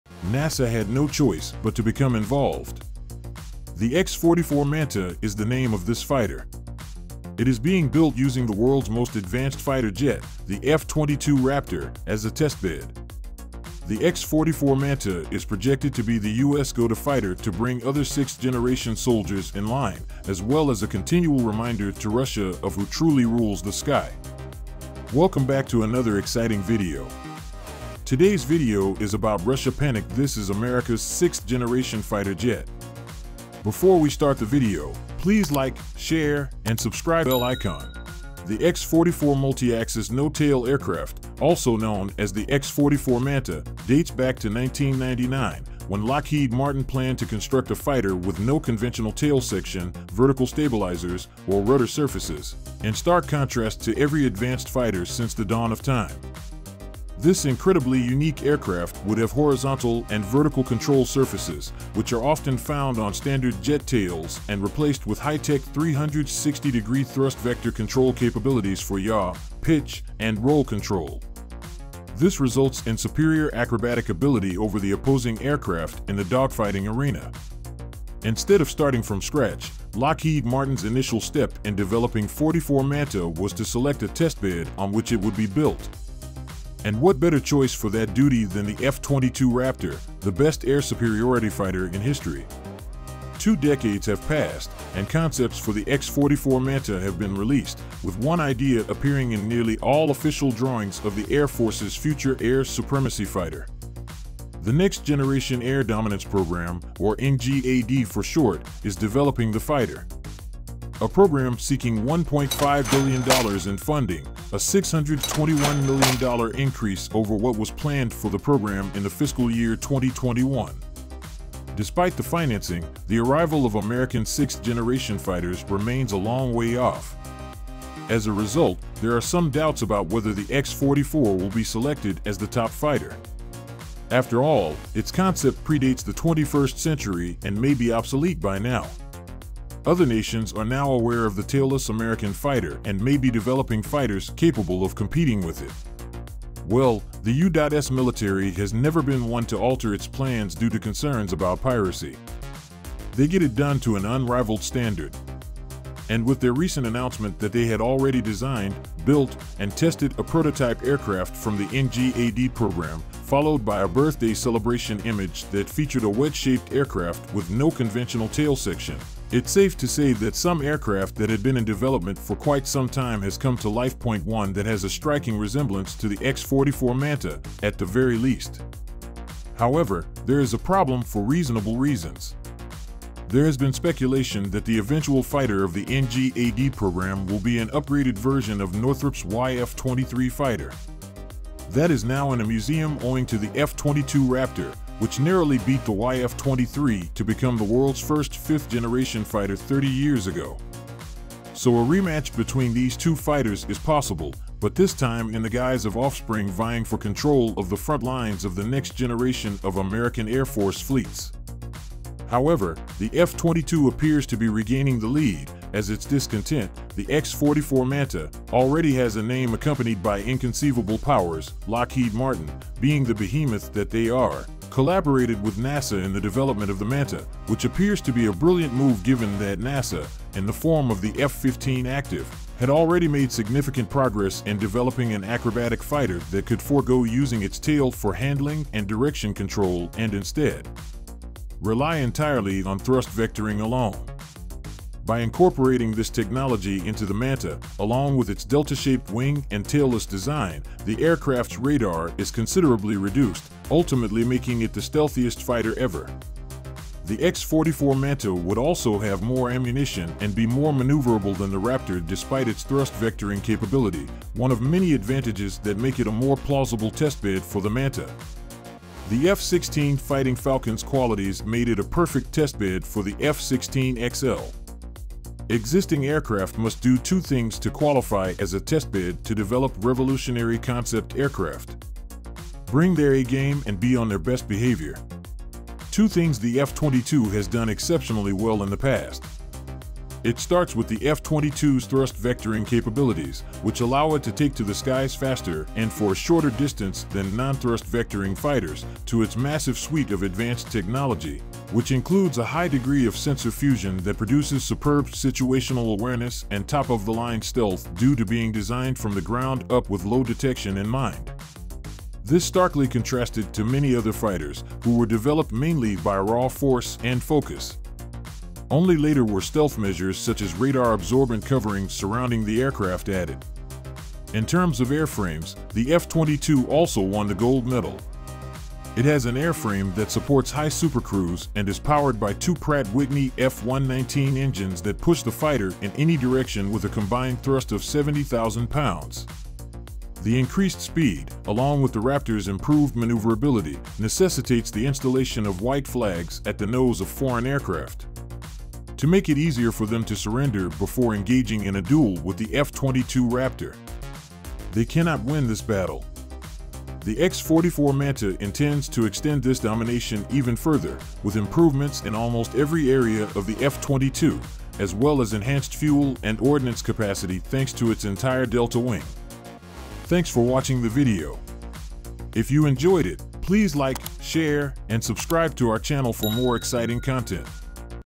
Russia Panic: This is America’s 6th Generation Fighter Jet
NASA had no choice but to become involved. The X-44 Manta is the name of this fighter. It is being built using the world's most advanced fighter jet, the F-22 Raptor, as a testbed. The X-44 Manta is projected to be the US go-to fighter to bring other sixth-generation soldiers in line, as well as a continual reminder to Russia of who truly rules the sky.